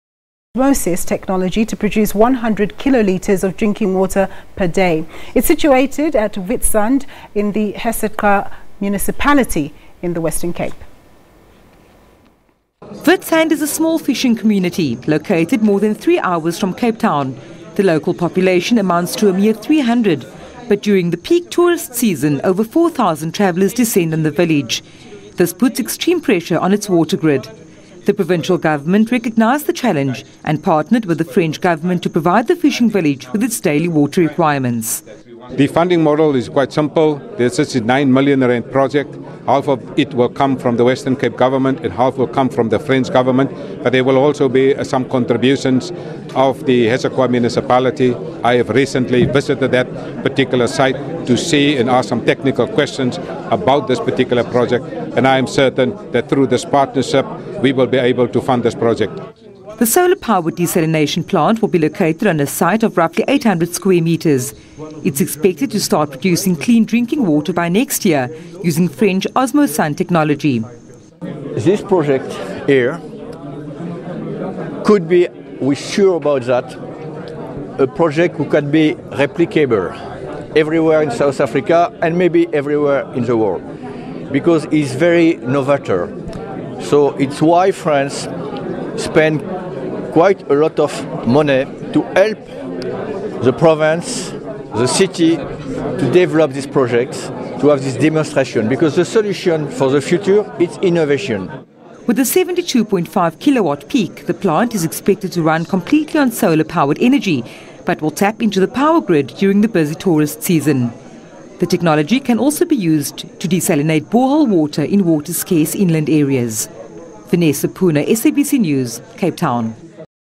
South Africa's 1st solar powered desalination plant kicks start
South Africa's first solar powered desalination plant is in production.
The plant will make use of reverse osmosis technology to produce 1-hundred kilolitres of drinking water per day. Its situated at Witsand in the Hessequa Municipality in the Western Cape.